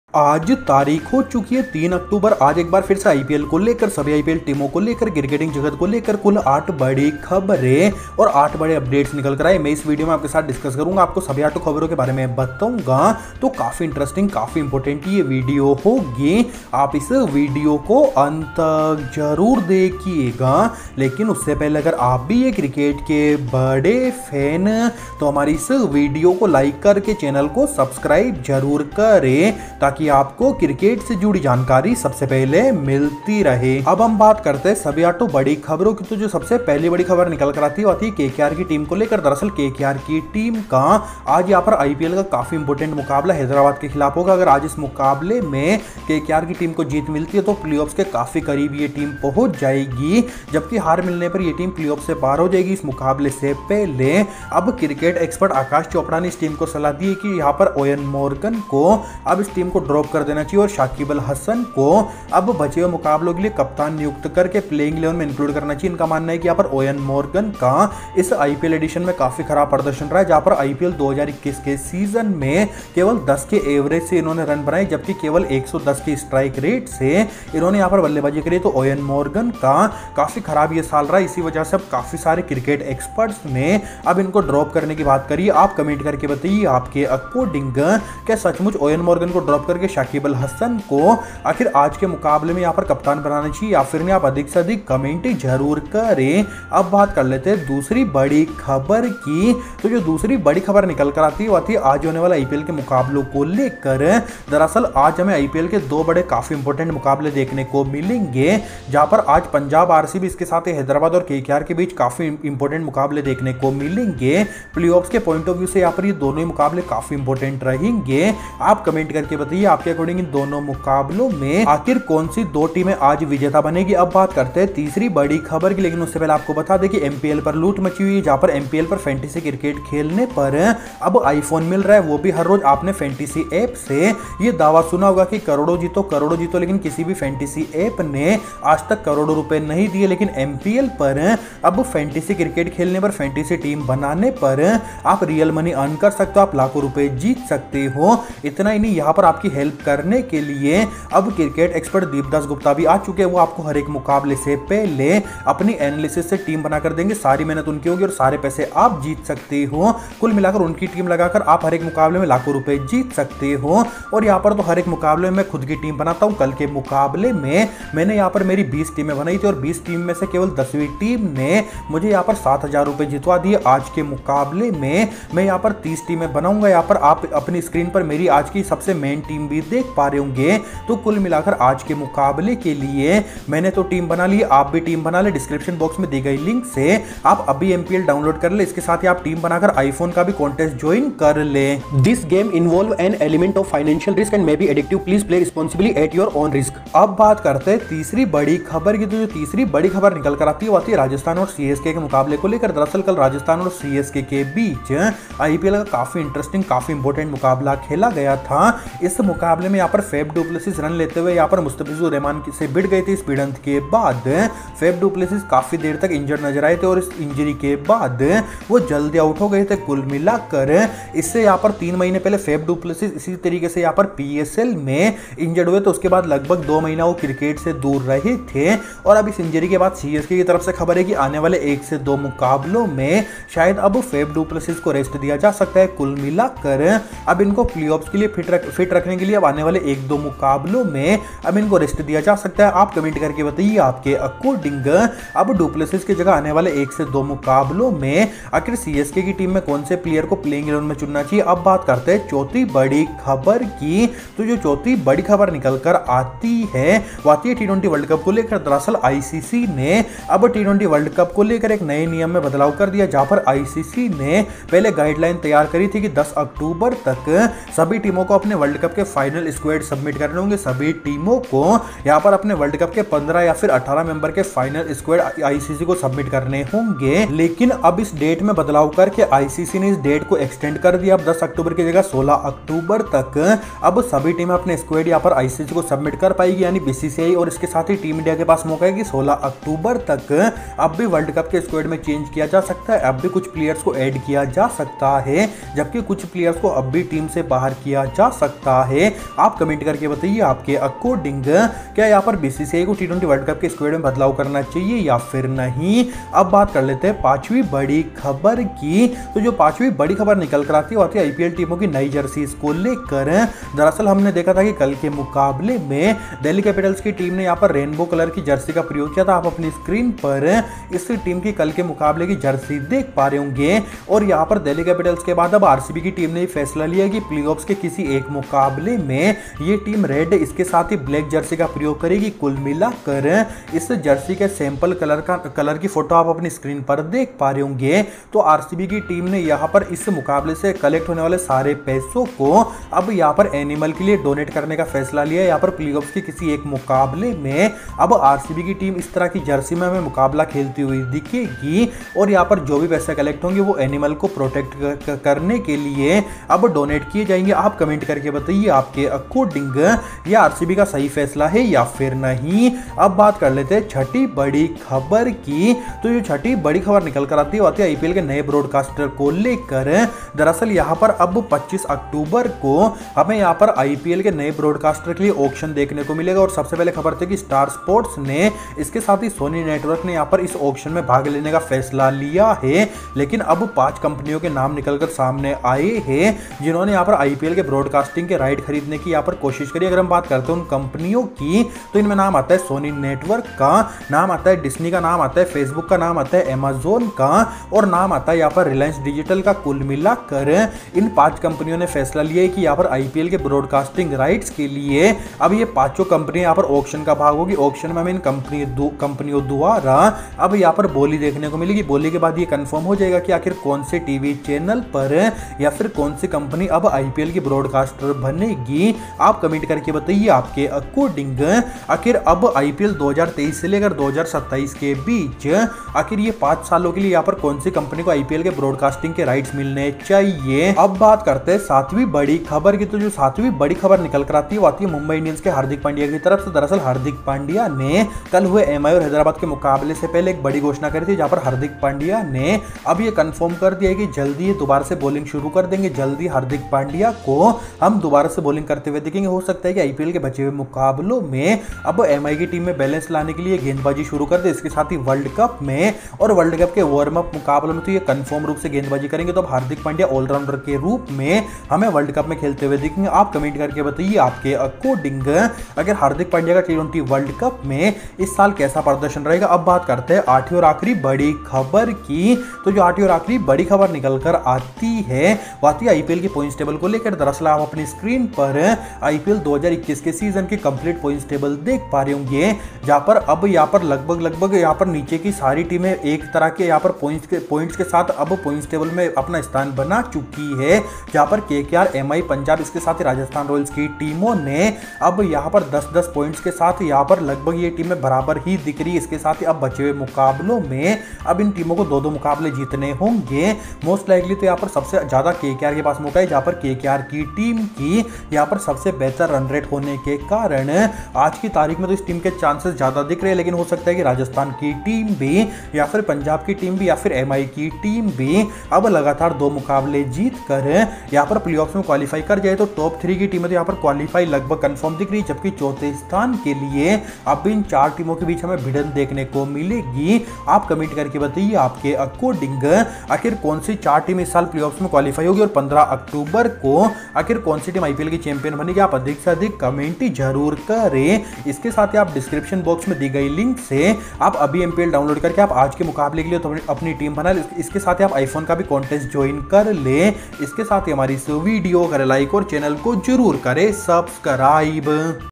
IPL 2021 - 8 Big News for IPL on 3 Oct (KKR New Captain vs SRH, RCB New Jersey for the Playoffs, MI)
Download the MPL App Now and Make your Fantasy Teams

Watch 8 Big News for IPL on 3 Oct (KKR New Captain vs SRH, RCB New Jersey for the Playoffs, MI, IPl Points Table)